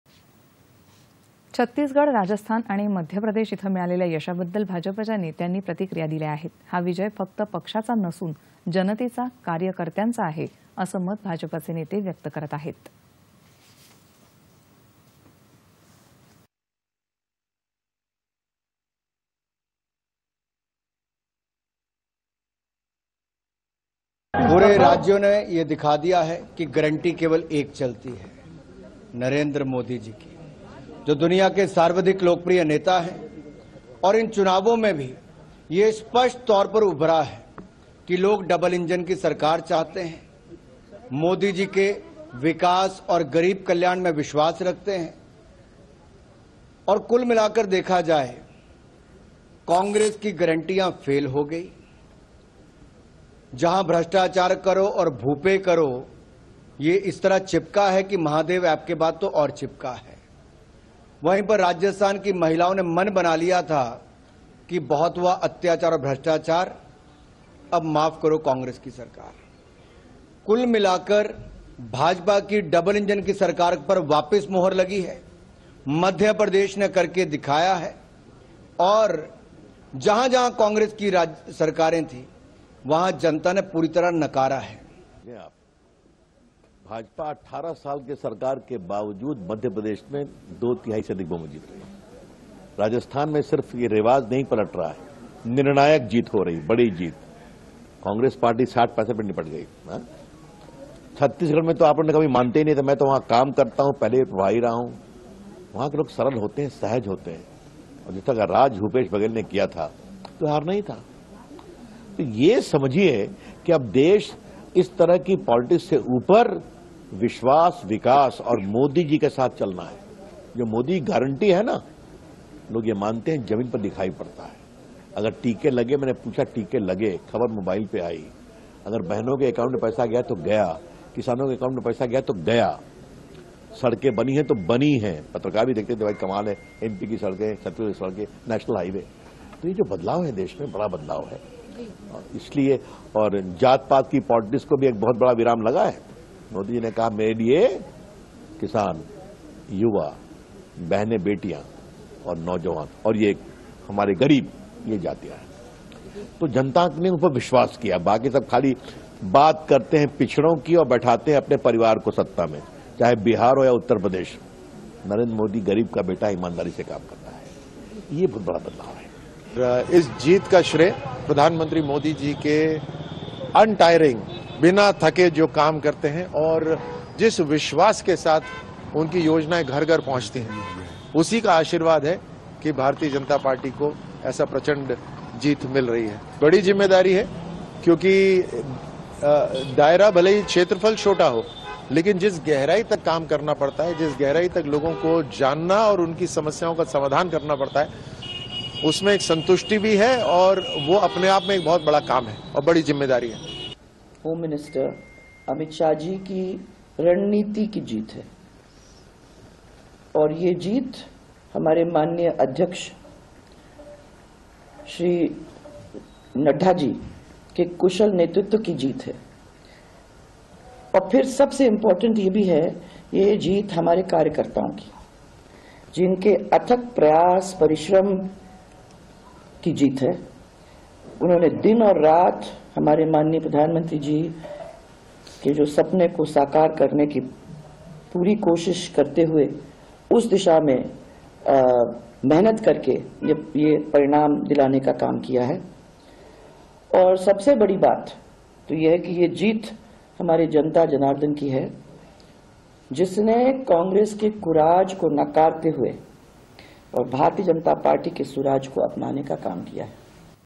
छत्तीसगड, राजस्थान,आणि मध्य प्रदेश इथं मिळालेल्या यशाबद्दल भाजपाच्या नेत्यांनी प्रतिक्रियादिल्या.
दि.०३.१२.२०२३ :
      छत्तीसगड, राजस्थान, आणि मध्य प्रदेश इथं मिळालेल्या यशाबद्दल  भाजपाच्या नेत्यांनी   प्रतिक्रिया दिल्या आहेत. हा विजय फक्त पक्षाचा नसून जनतेचा  - कार्यकर्त्यांचा आहे असं मत भाजपाचे नेते व्यक्त करत आहेत.
-- सिंक --- -.
1.अनुराग ठाकूर, केंद्रीय मंत्री.
2.रविशंकर प्रसाद, प्रवक्ते, भाजपा.
3.राज्यवर्धनसिंह राठोड, भाजपाचे नेते.
4.वसुंधराराजे सिंधिया, भाजपा नेत्या.

8.30am | 11.00am | 1.00pm | 4.30pm | 7.00pm | 9.30pm